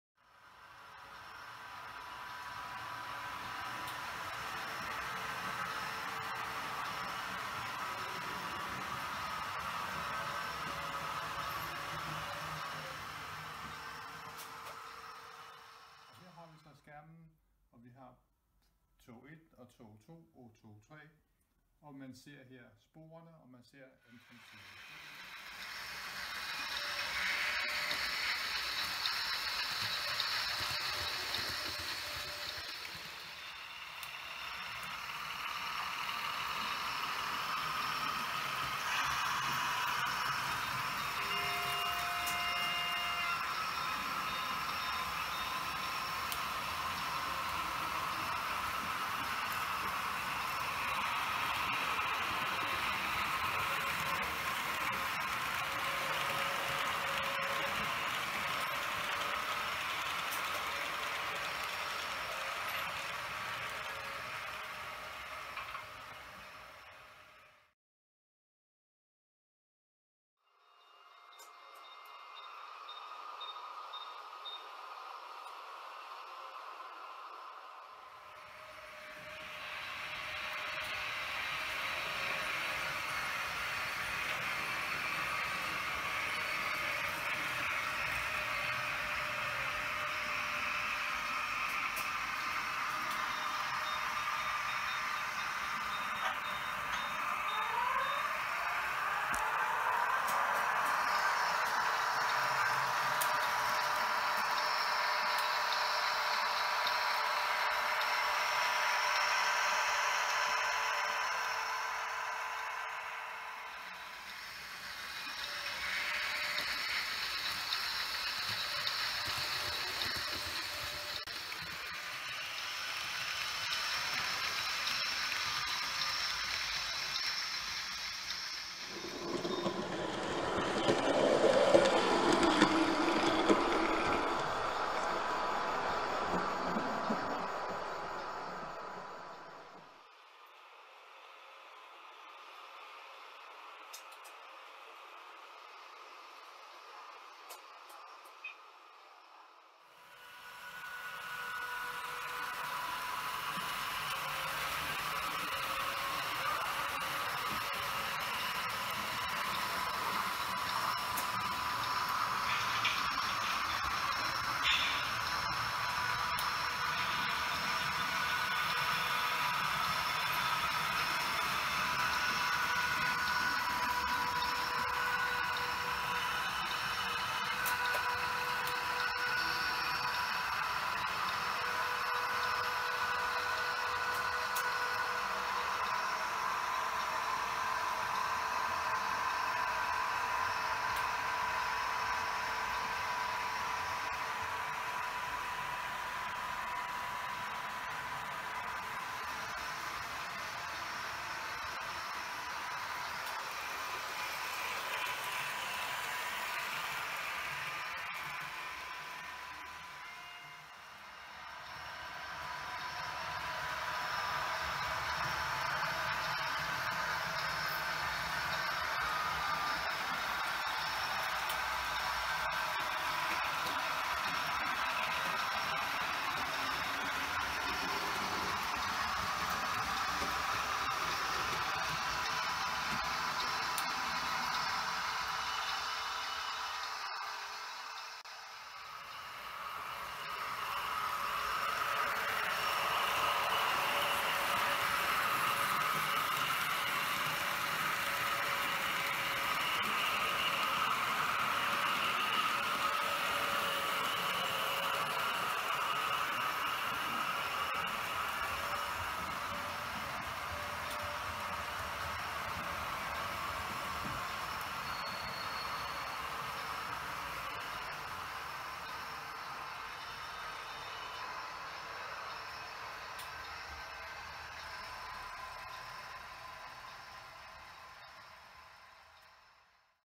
Real time Arrival Board for small and GIANTS people
In Kælderkøbing there are now Real-time Arrival Boards for both small and LARGE people. Technical description of the solution can be found